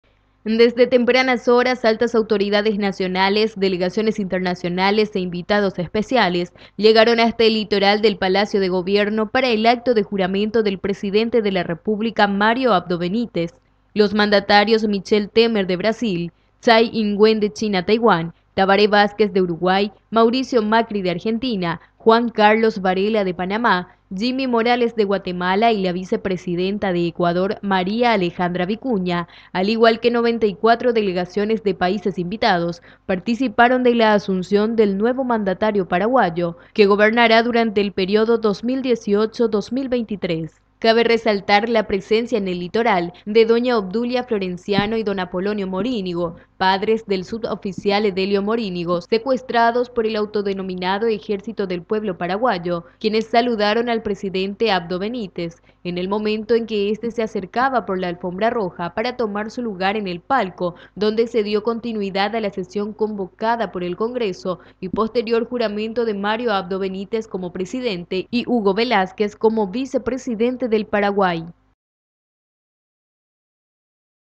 TRASPASO PRESIDENCIAL
Mandatarios y comitivas extranjeras presentes en la toma de mando